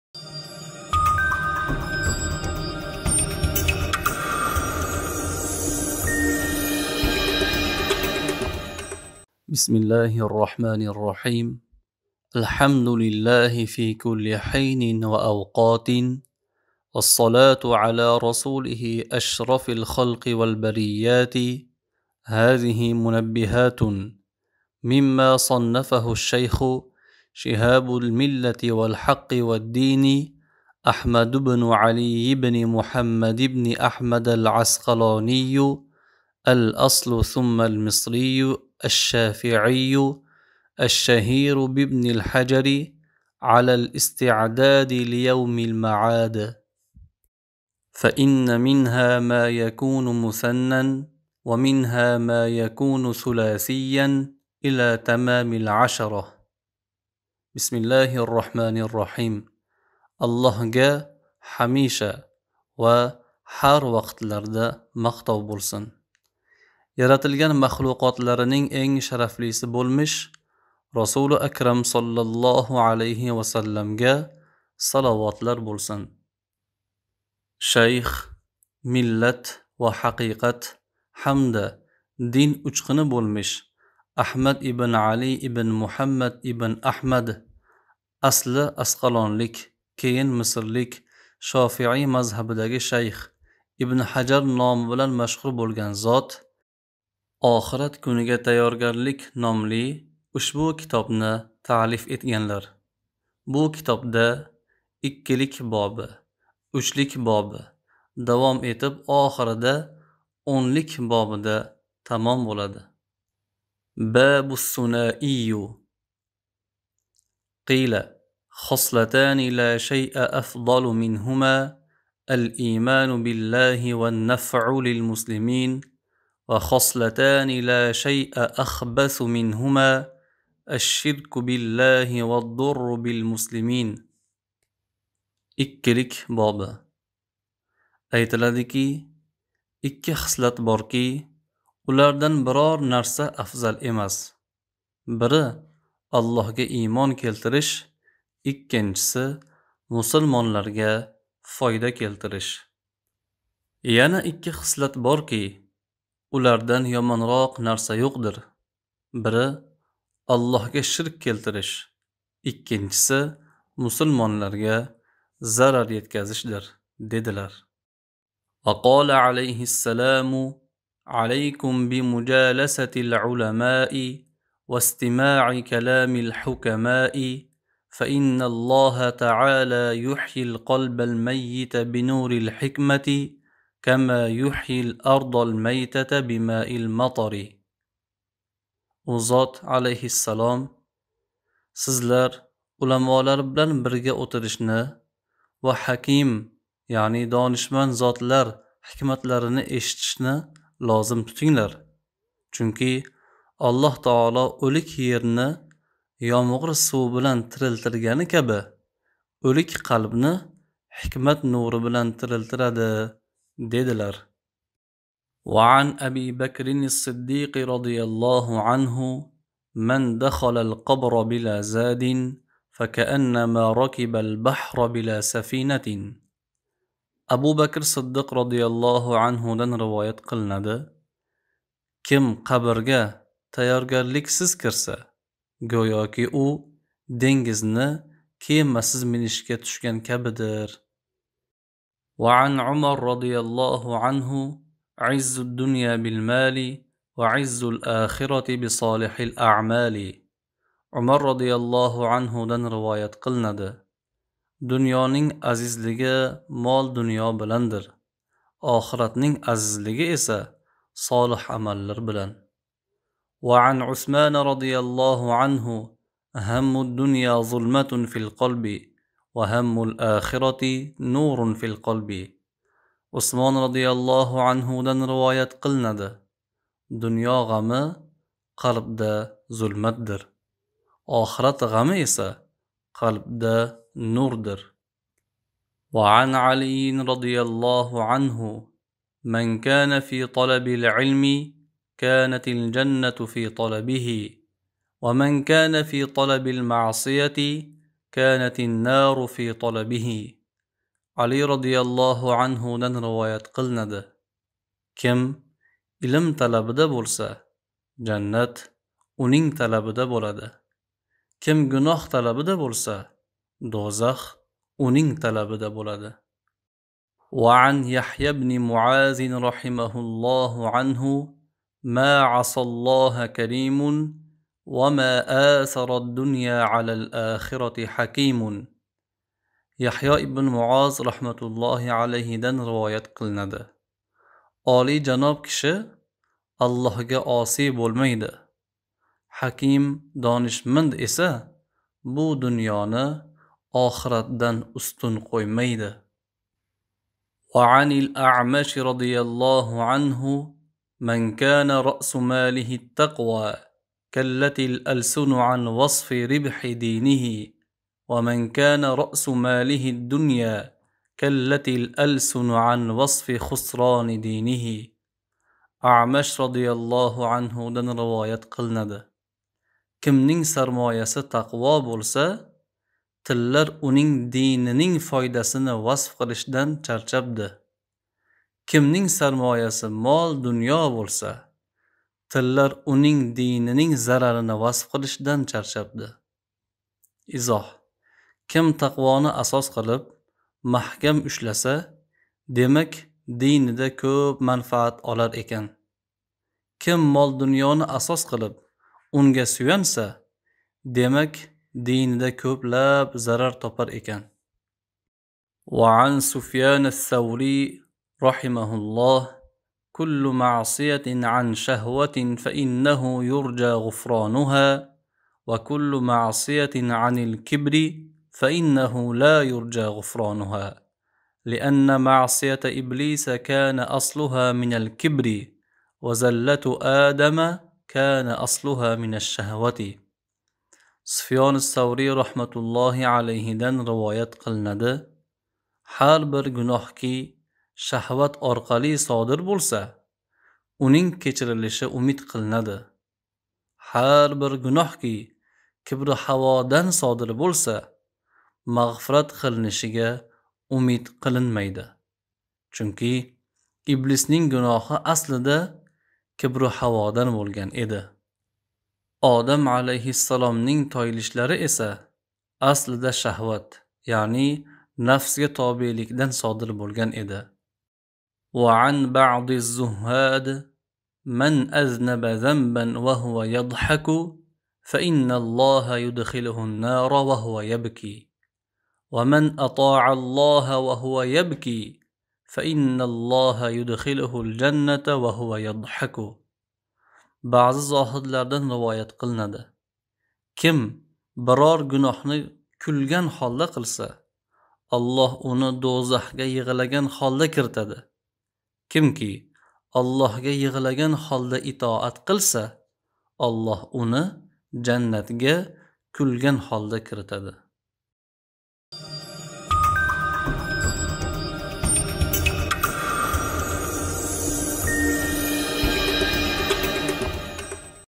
2 OXIRAT KUNIGA TAYYORGARLIK | ABDUSAMAD TOJIDDINOV
Mir Arab o'rta maxsus islom bilim yurti mudarrisi